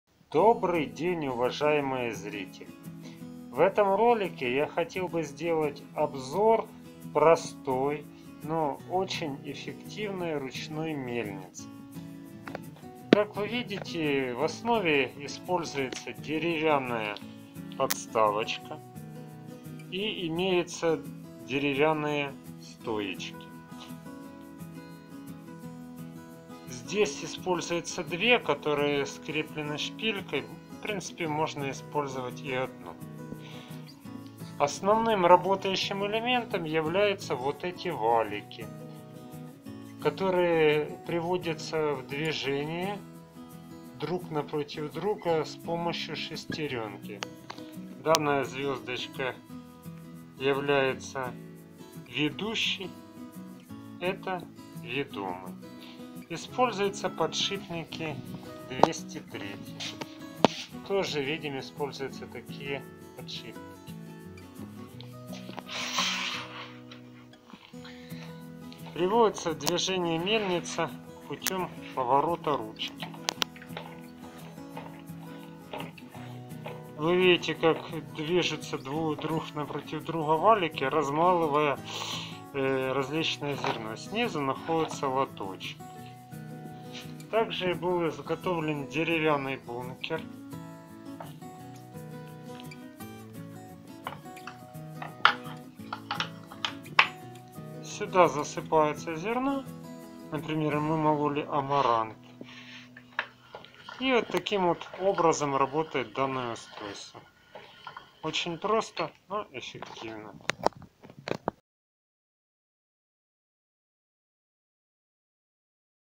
Ручная мельница своими руками. Простая и эффективная конструкция
В ролике делается обзор простой, но эффективной зерномолки. Ее можно использовать для получения цельнозерновой муки. Также перемалывали амарант и другие зерна. Помол осуществляется двумя валиками, которые вращаются навстречу друг другу. В конструкции используются четыре 203 подшипника.